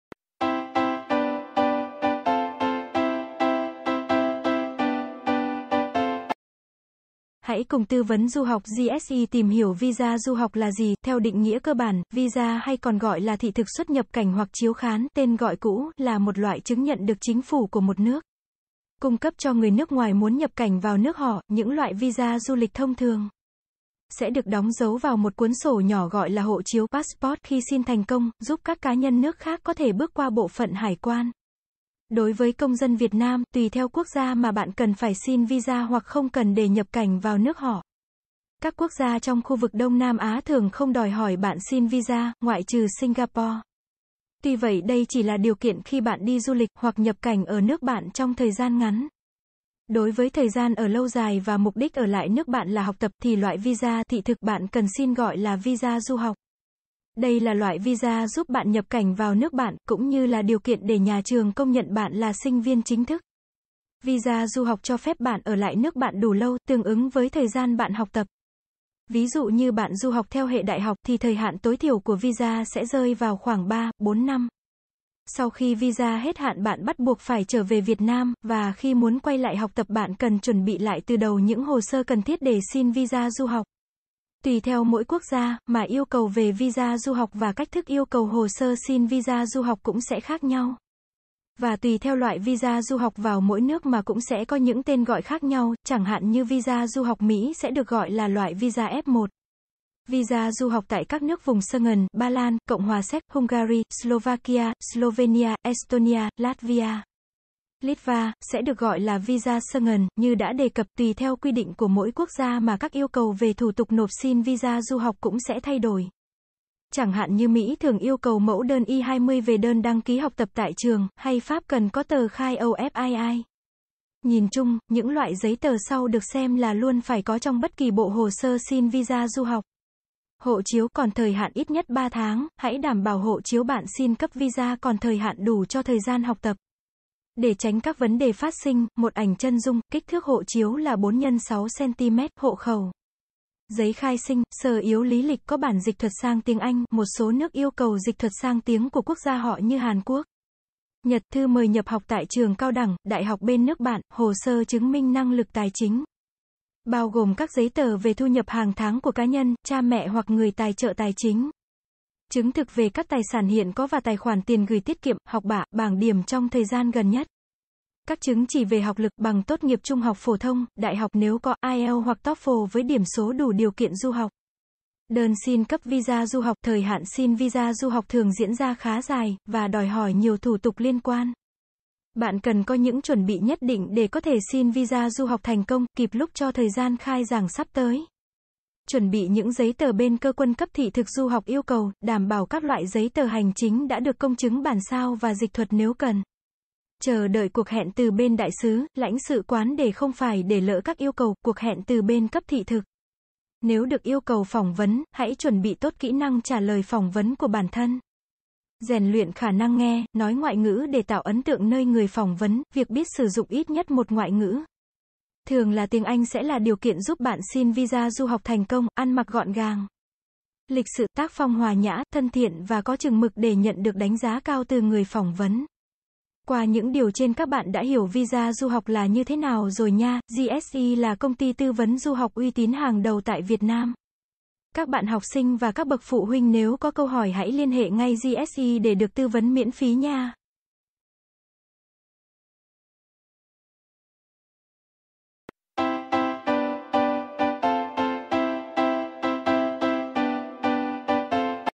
Visa du học là gì ?
Theo định nghĩa cơ bản, visa hay còn gọi là thị thực xuất nhập cảnh hoặc chiếu khán (tên gọi cũ) là một loại chứng nhận được Chính phủ của một nước, cung cấp cho người nước ngoài muốn nhập cảnh vào nước họ. Những loại visa du lịch thông thường, sẽ được đóng dấu vào một cuốn sổ nhỏ gọi là hộ chiếu (Passport) khi xin thành công, giúp các cá nhân nước khác có thể bước qua bộ phận hải quan.